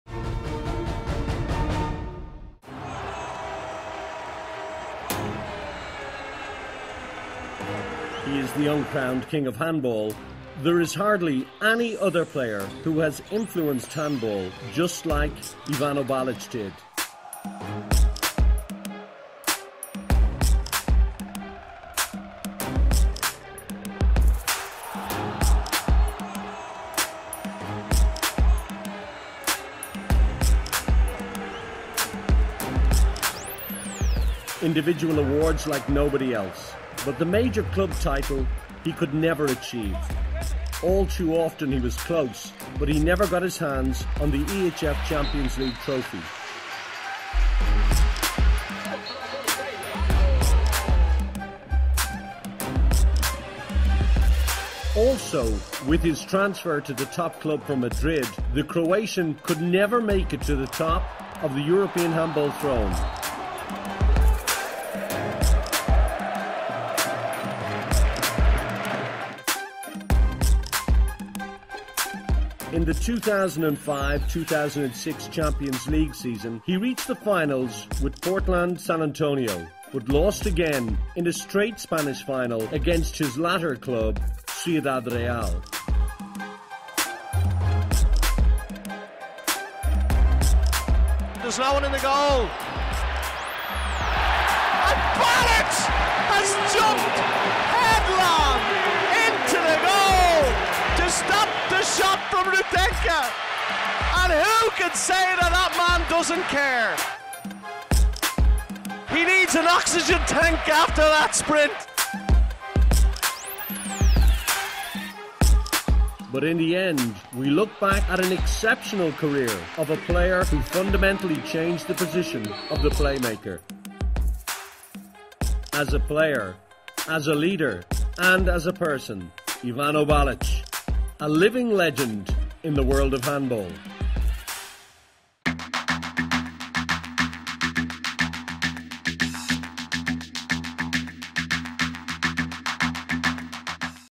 Throwback Thursday - Ivano Balic
Is Ivano Balic the greatest player to never win the EHF Champions League? We look back at the career of handball's 'Mozart' and his finest moments in Europe's elite club competition with the likes of Portland San Antonio, Zagreb and Atletico Madrid.